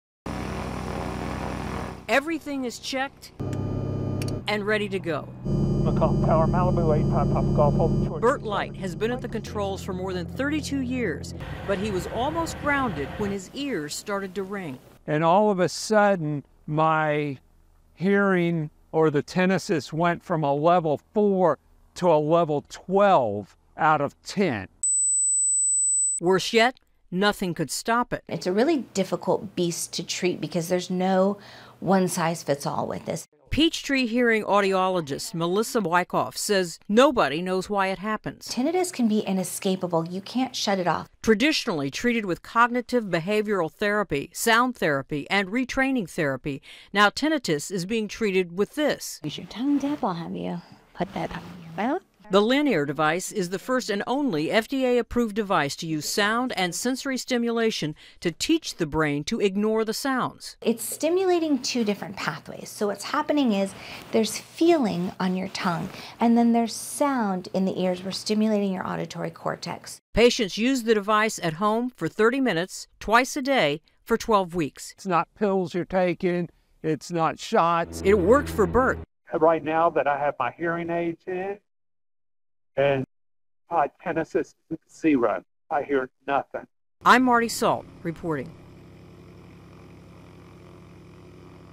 SILENCING THE SOUNDS: BREAKTHROUGH TINNITUS TREATMENT
It’s a loud, uncontrollable sound that only you can hear. Now with the help of a new FDA approved device you can shut off the noise in your brain